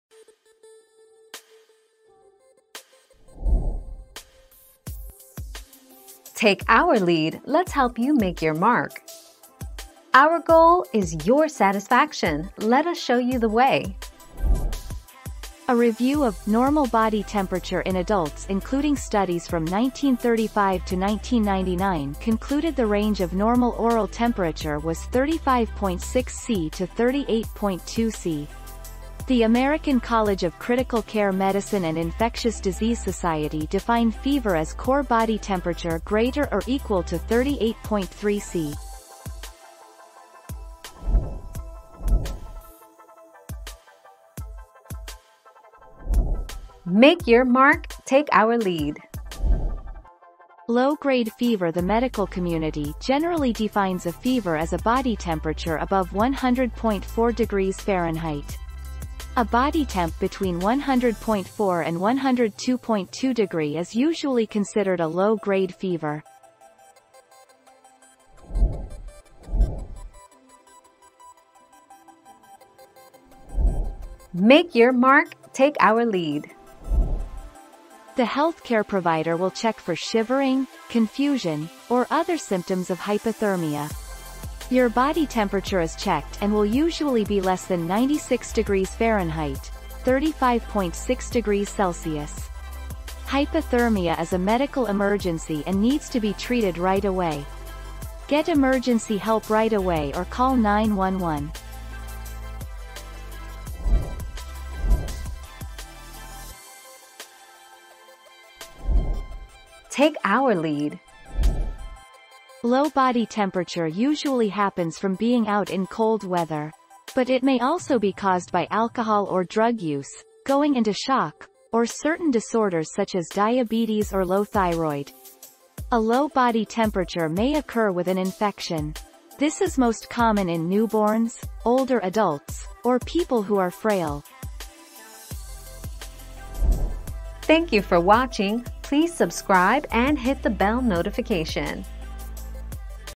Is 35.6 a normal temperature for adults?
00:00 - Is 35.6 a normal temperature for adults?
00:43 - Is a low temp as bad as a high temp?
01:10 - Is a temperature of 35.6 Low?
01:45 - What does a low temperature mean in adults?

Laura S. Harris (2021, March 30.) Is 35.6 a normal temperature for adults?
    AskAbout.video/articles/Is-35-6-a-normal-temperature-for-adults-204012